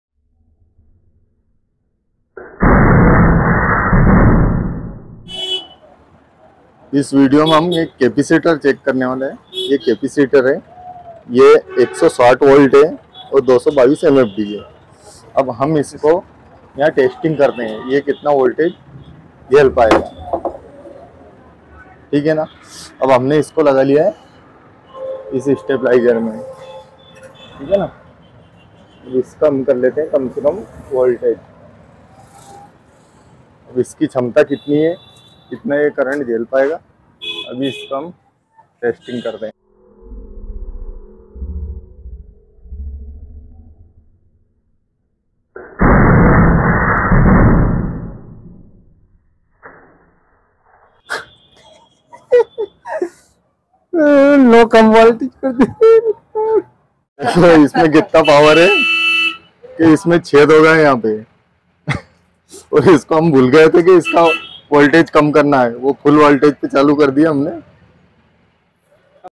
desi jugaad kripya Ghar per na kare no try for home
humne 160v capacitor
#220 MfD ka testing
yahan Kitna voltage jal sakta hai